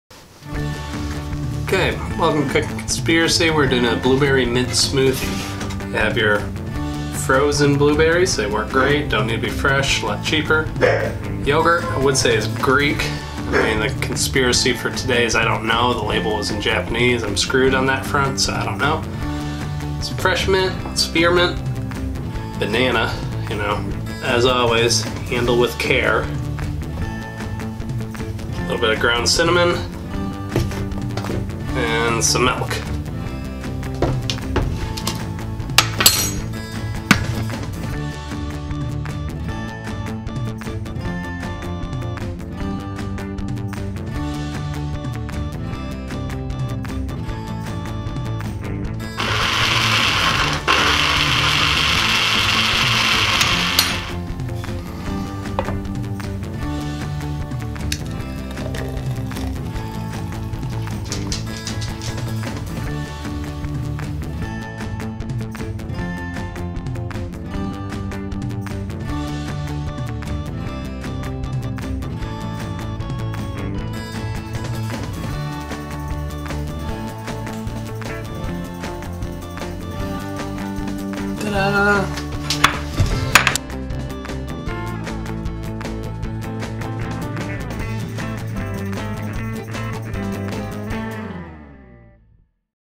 Blueberry Mint Smoothie: Delicious Blueberry Smoothie with Banana and Mint
Easy fresh smoothie made for quick healthy breakfast or snacks.
Ingredients:
Blueberries 1 cup
Banana 1 banana
Mint 4-5 leaves

Add them to a blender and blend to the consistency that you like.
Enjoy this quick and easy smoothie recipe by cooking conspiracy.  Super healthy and easy to make.